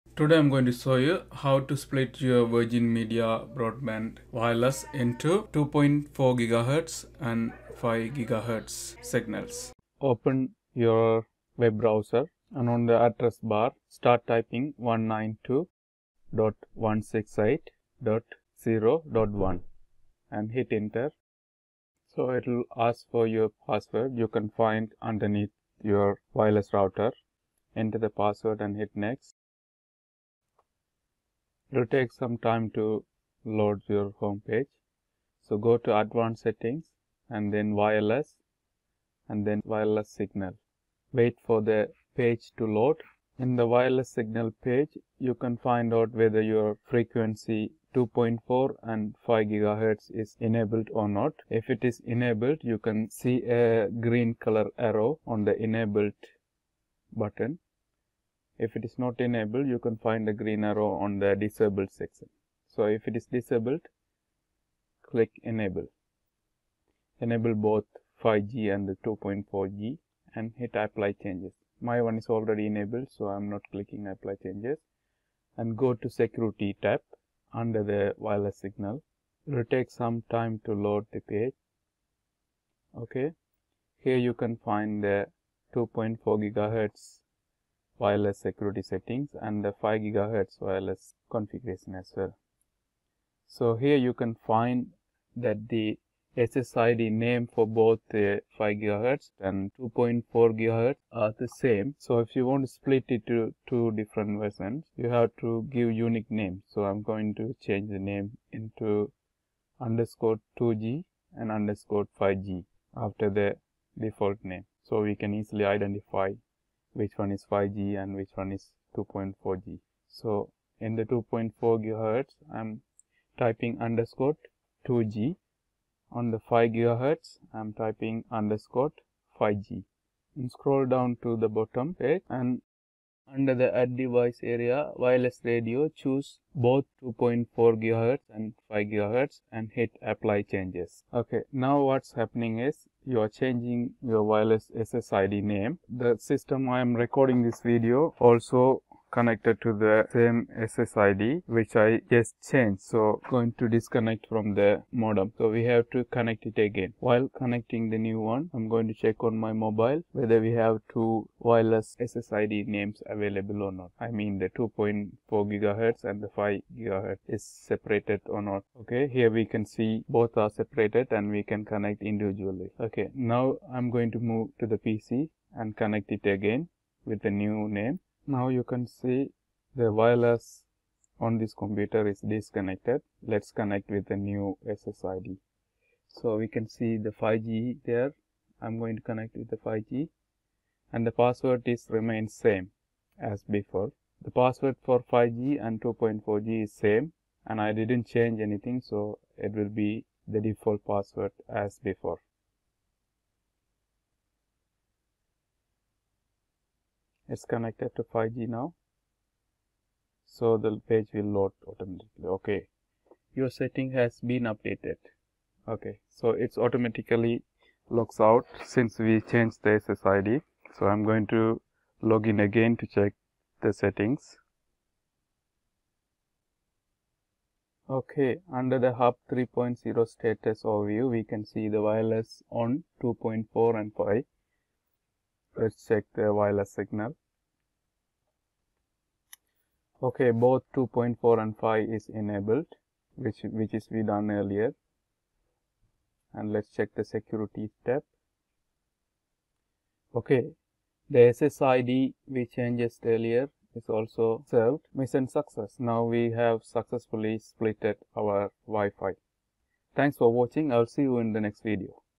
Virgin Media Broadband Wifi split into Dual Band 2.4GHz and 5GHz | IP camera Wifi 2.4GHz enable
Dual Band Hardware recombination
In this simple tutorial i'm showing how to split your wireless band into 2.4GHz and 5GHz dual bands.
By default Virgin Wifi is set to Combined band which is fast and efficient. But some devices such as IP camera's only support 2.4GHz so we have to split the wifi signal in order to connect those devices.

2.4GHz signal is long range so its good to have one since we also having the Fastest 5GHz band as well.